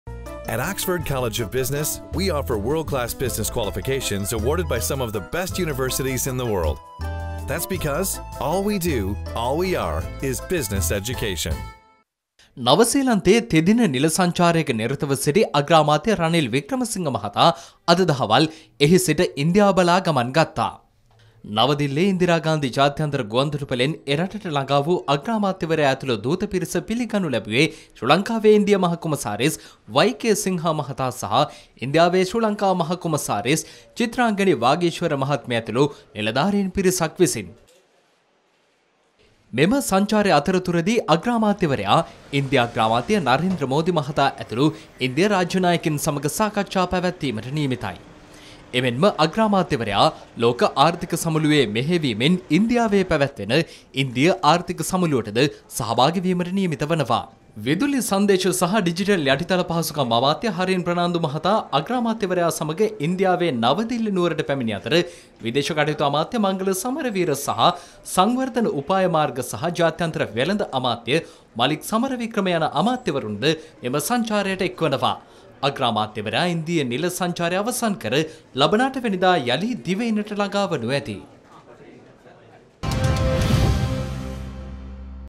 Sri Lankan PM to meet Narendra Modi during India visit
අගමැති ඉන්දියාවට පා තබයි